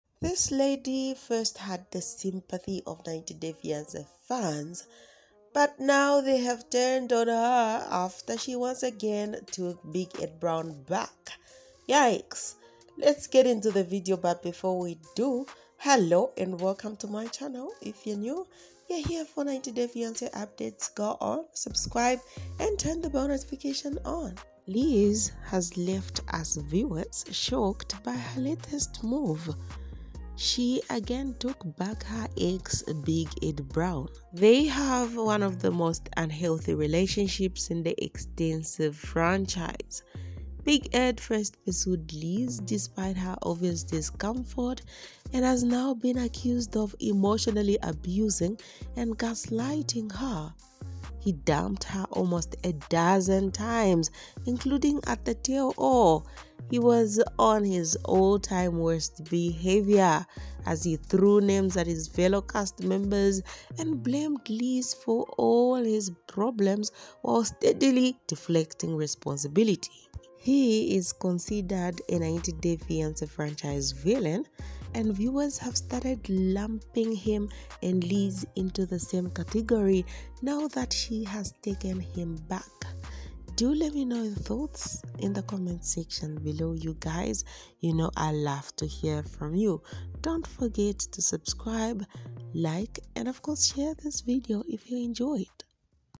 shocking update! liz shocks her fans
Liz Woods first had the sympathy of 90 Day Fiancé franchise fans, but now viewers have turned on her after she once again took Big Ed Brown back.